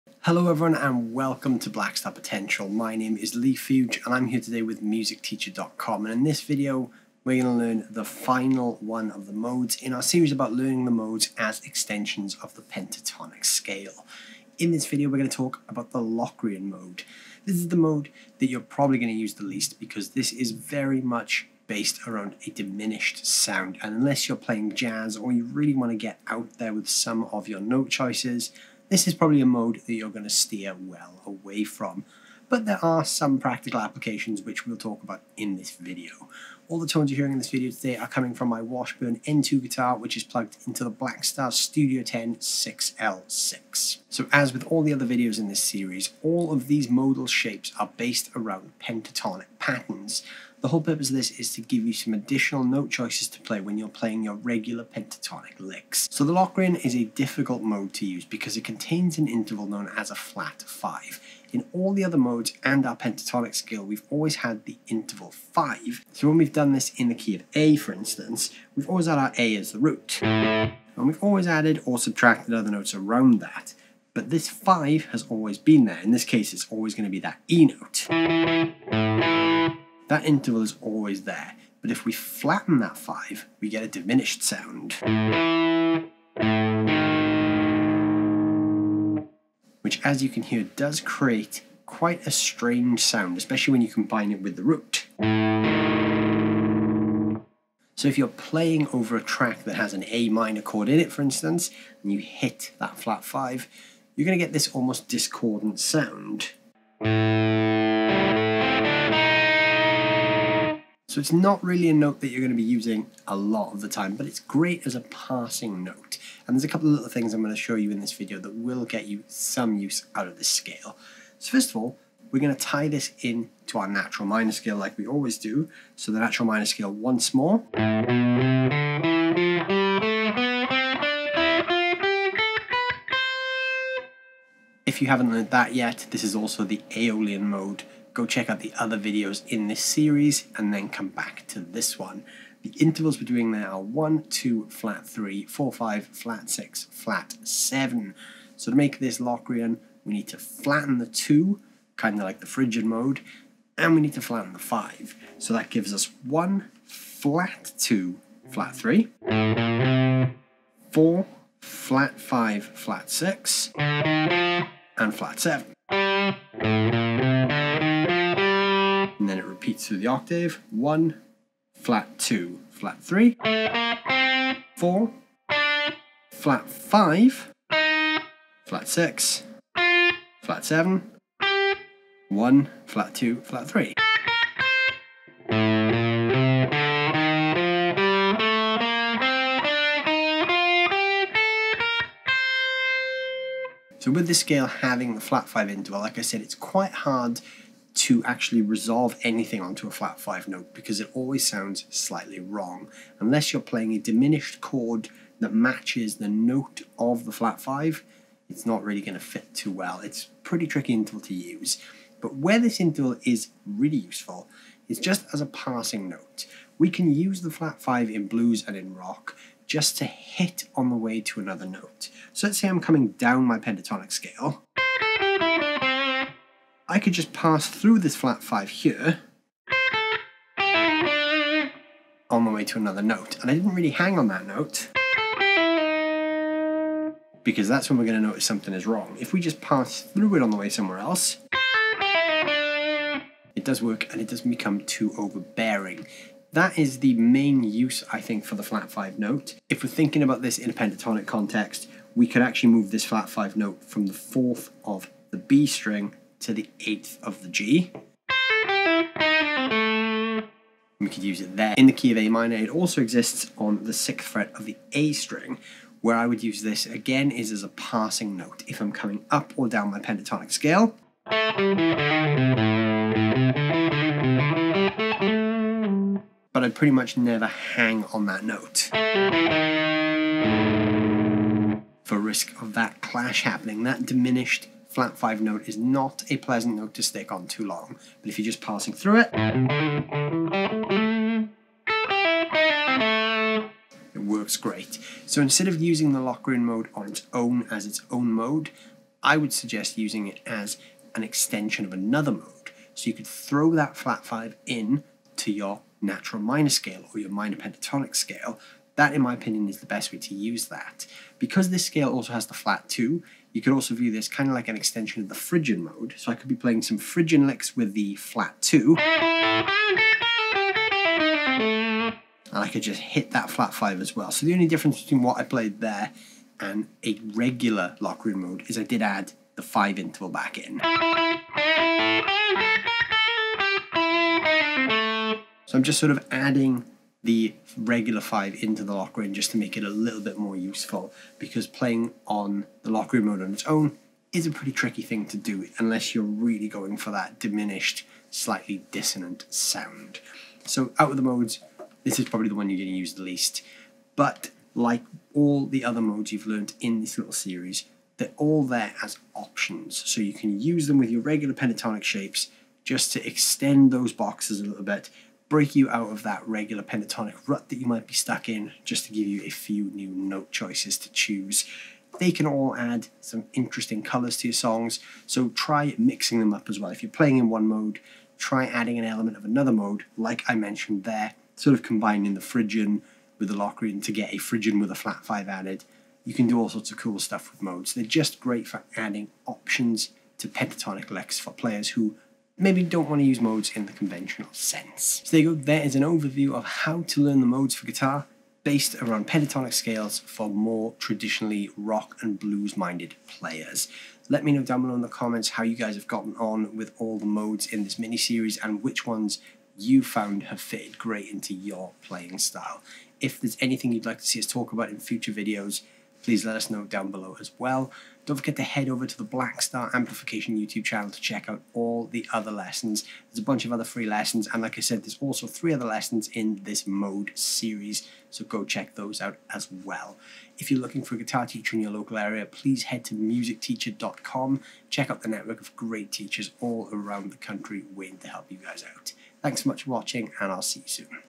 Learning Modes | Part 4 | The Diminished Mode | Blackstar Potential Lesson
Do you want to learn the diminished mode? In this week's Blackstar Potential lesson with @Leigh Fuge of MusicTeacher.com we'll be looking at exactly that! Let us know how you got on!